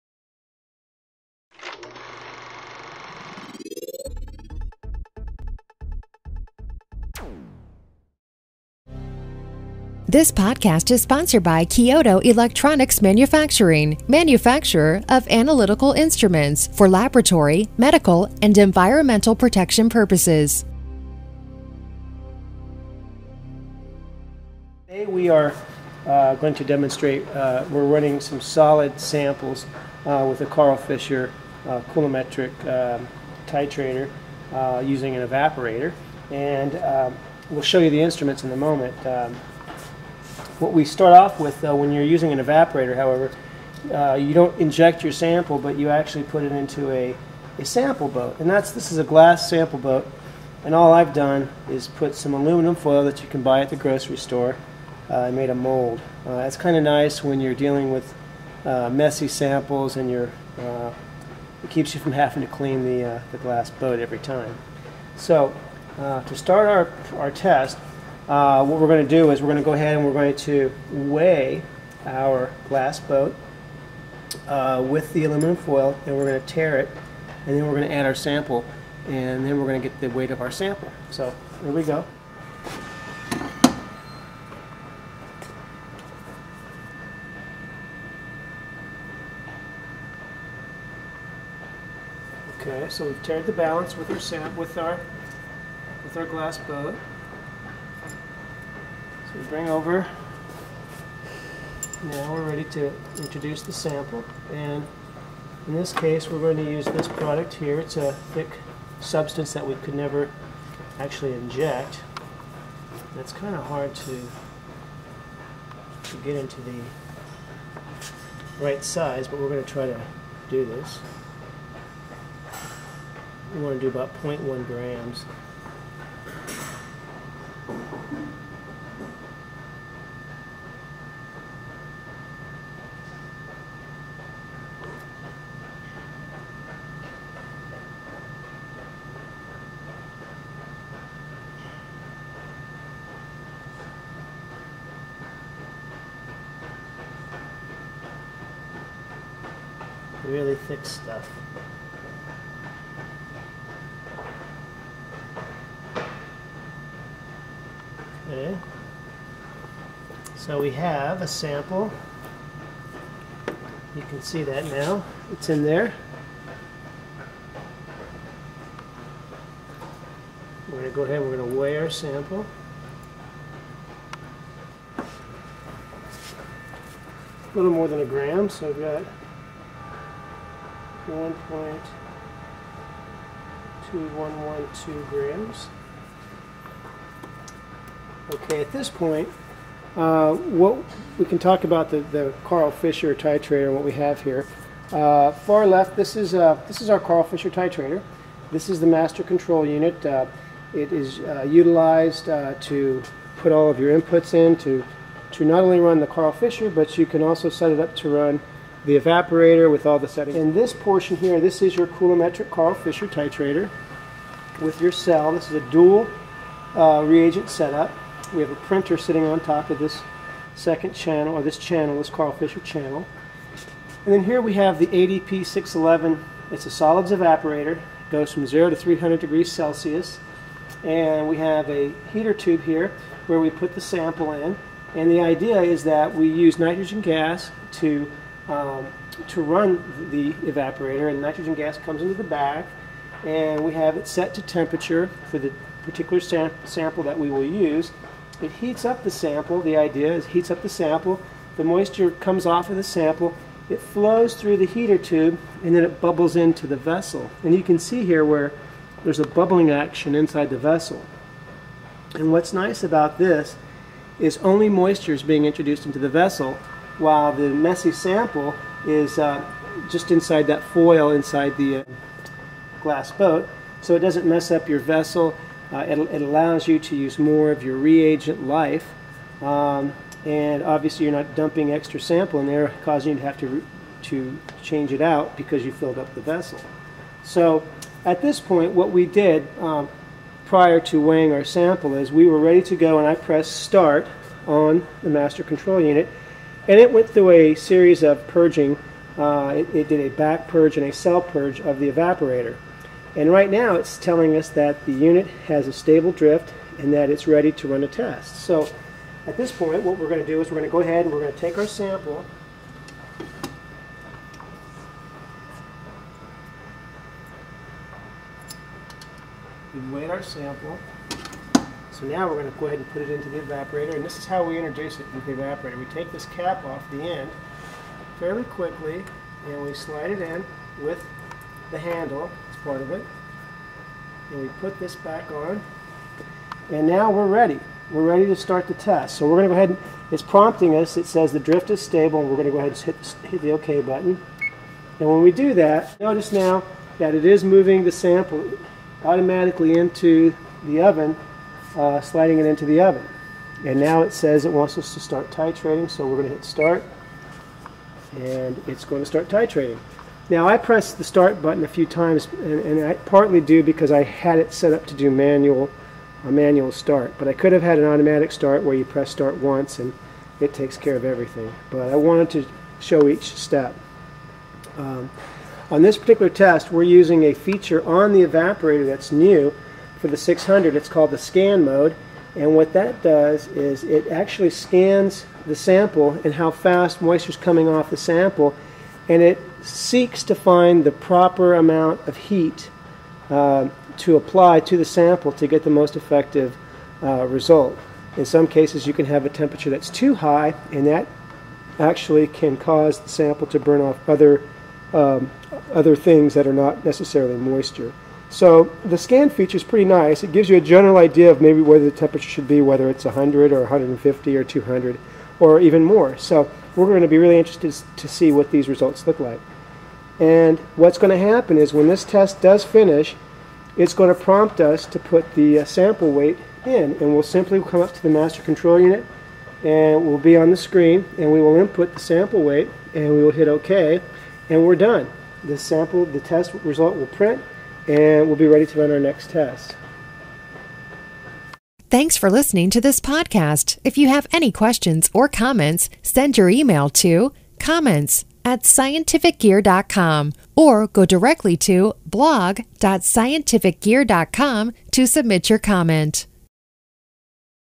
Karl Fisher Titrator MKC610 and Solids Evaporator ADP611
This video demonstrates step by step how to weigh a sample and run a karl fisher titrator using a solids evaporator oven.